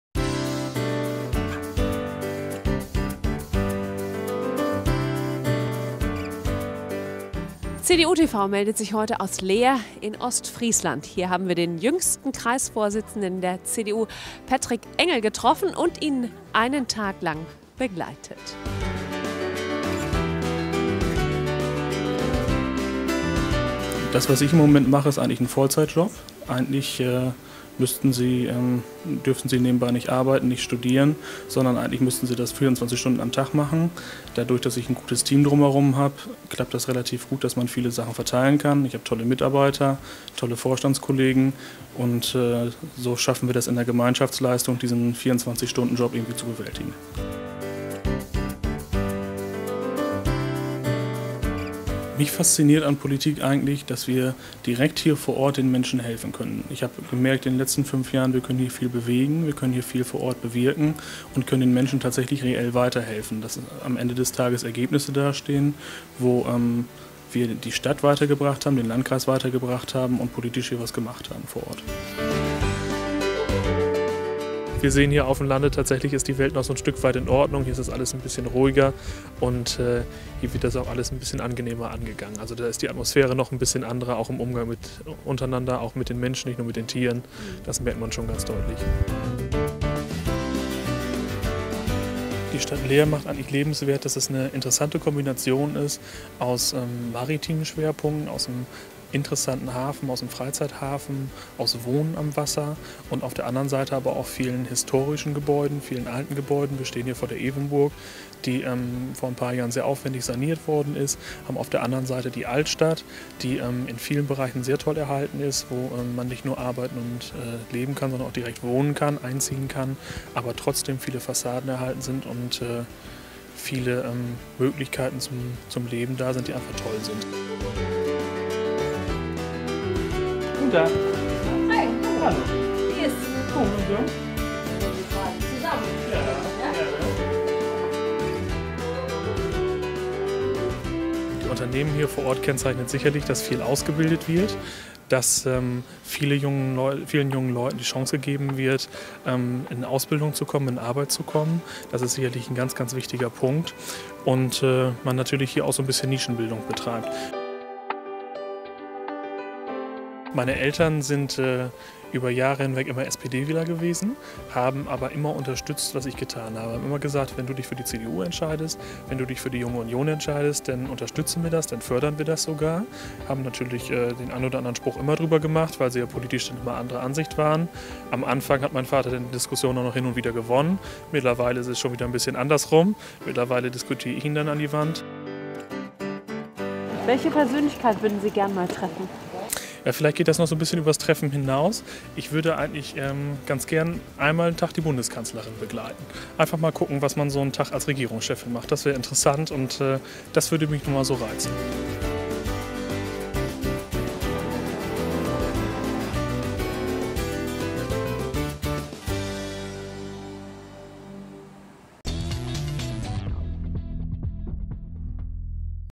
CDU.TV besucht den jüngsten CDU-Kreisvorsitzenden
CDU.TV besucht Patrick Engel, den jüngsten CDU-Kreisvorsitzenden, in seinem Verband in Leer (Ostfriesland)